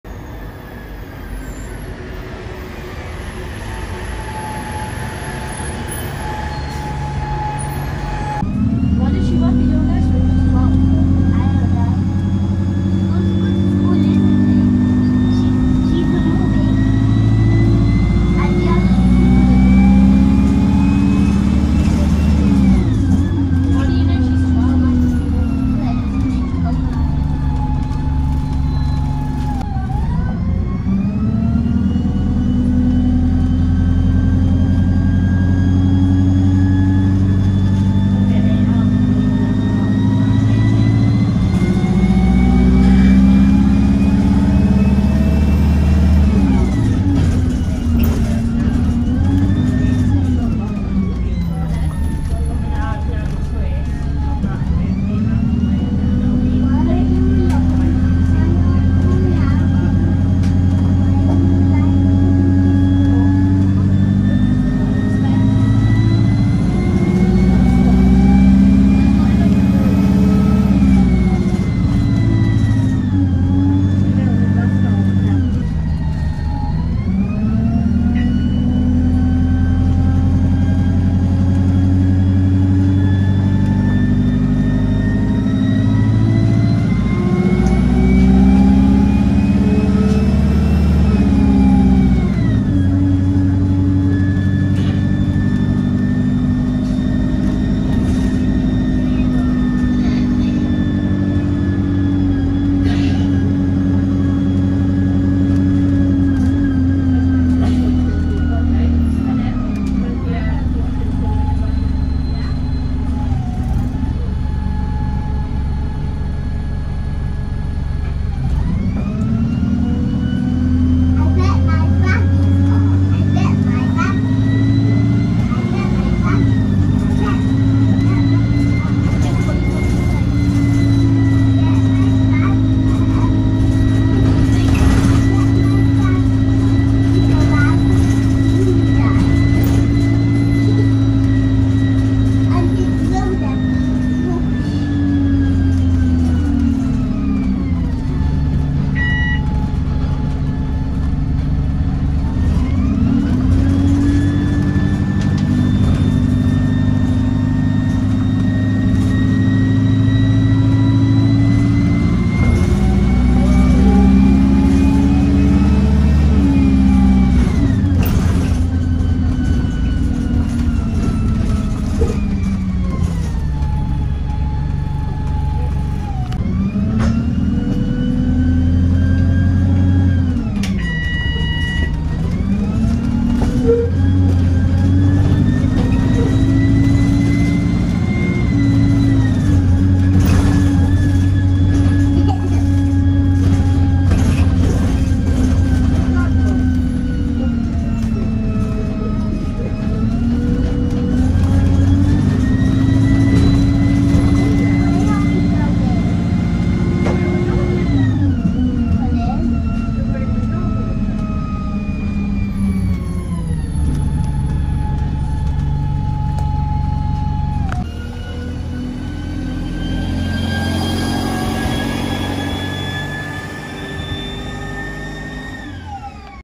Stagecoach Yorkshire: 36413 on Route 39 | ADL Enviro 200 | (UK Bus Ride #104)
Bus Ride of Stagecoach Yorkshire's Alexander Dennis Enviro 200 running on route 39 heading towards Birdholme

This was filmed on Monday 19th June 2023

Bus:
Stagecoach Yorkshire (Stagecoach in Chesterfield)
36413 YN11 FFD
2011 Alexander Dennis Enviro 200

Route:
39: Holme Hall (Cauldon Drive) to Birdholme (Grangewood Farm)
(via Loundsley Green, Chesterfield)